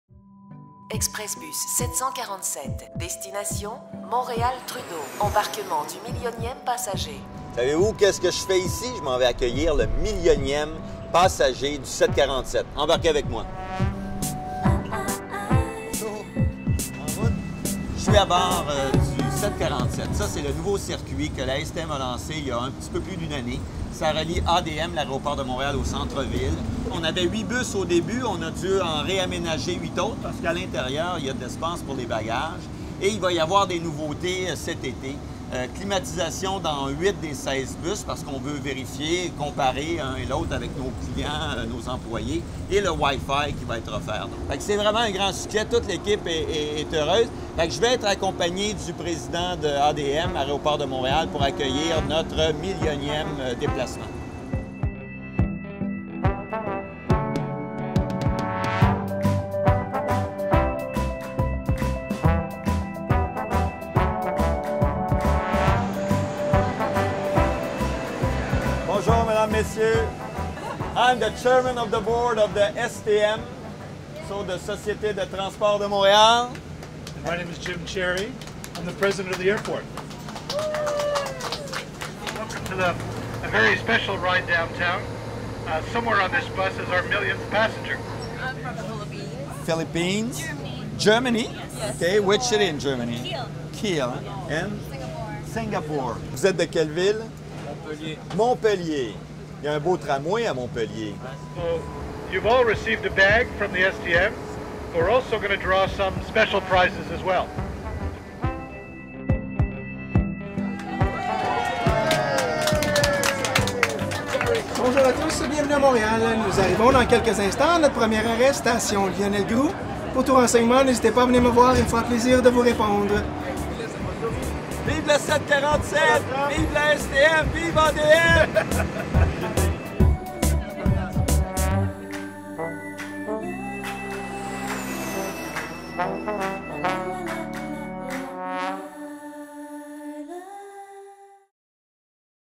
Le millionième passager de la ligne 747 Express !
Plus d'un million de passagers ont emprunté la ligne 747 Express Bus depuis un peu plus d'an après sa mise en service. Cette liaison directe entre le centre-ville de Montréal et l'Aéroport Montréal-Trudeau est forte d'un achalandage qui s'élève à quelque 3 500 déplacements par jour moyen de semaine en forte saison.